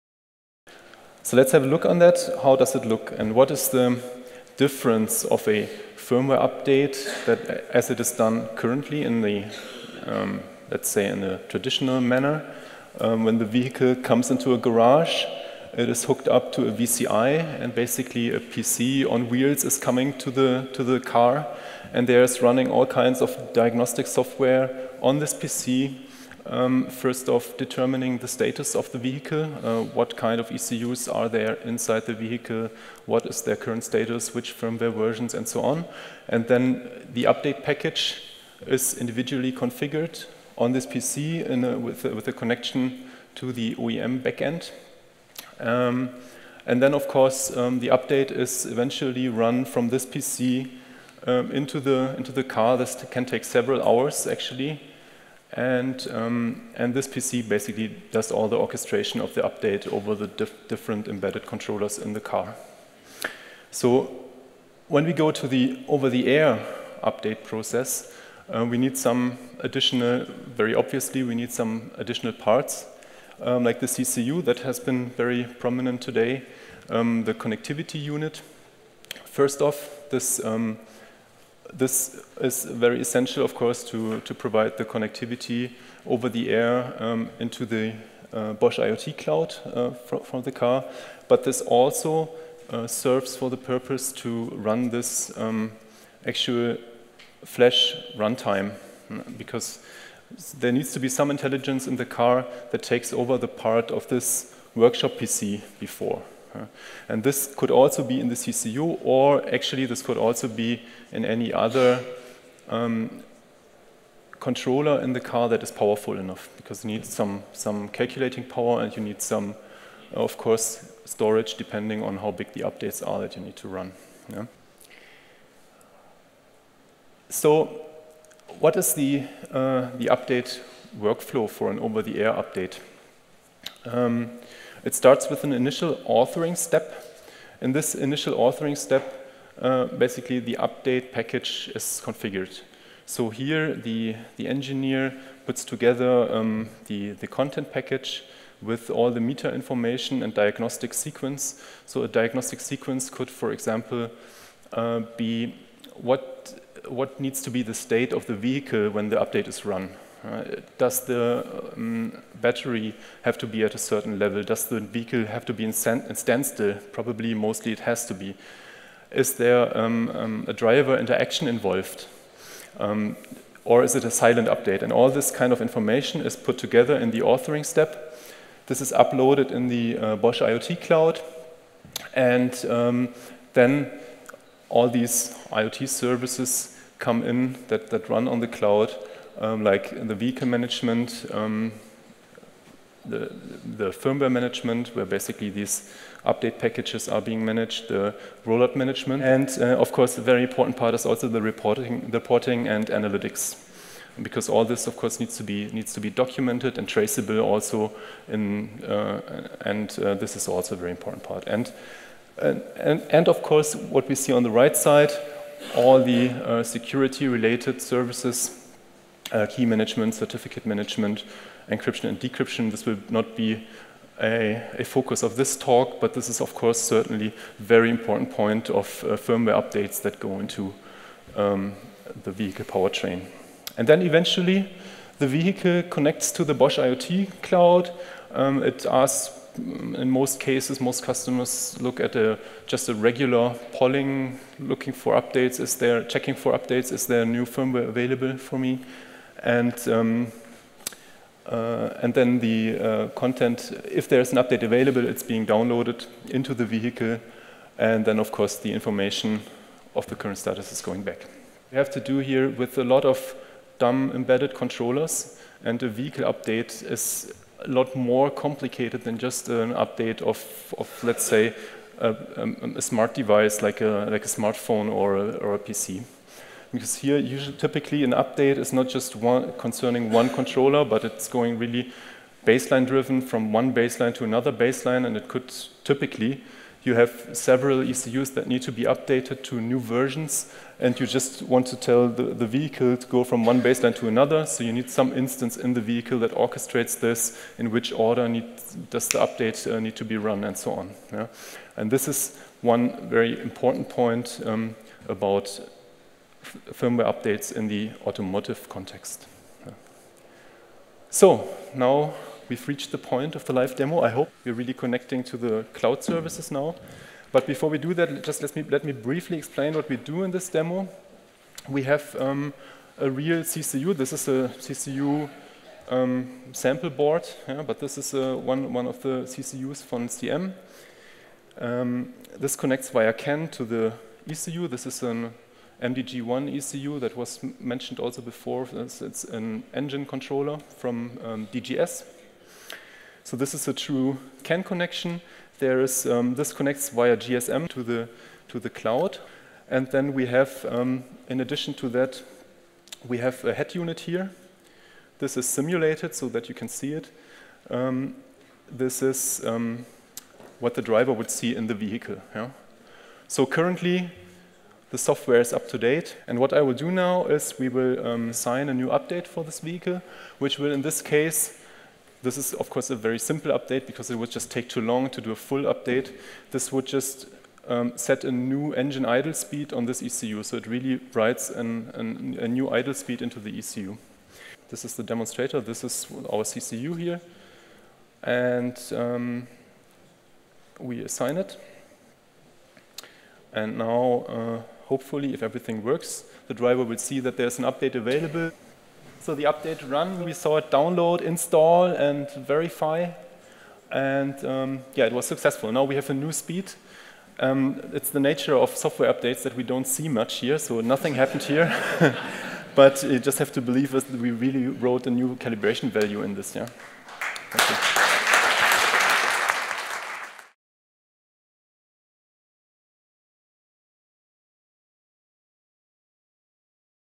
Firmware update over the air – insights and live demo
Firmware update in vehicles – what’s the difference between the traditional and the over the air approach? Learn more on this and experience a live demo in this video.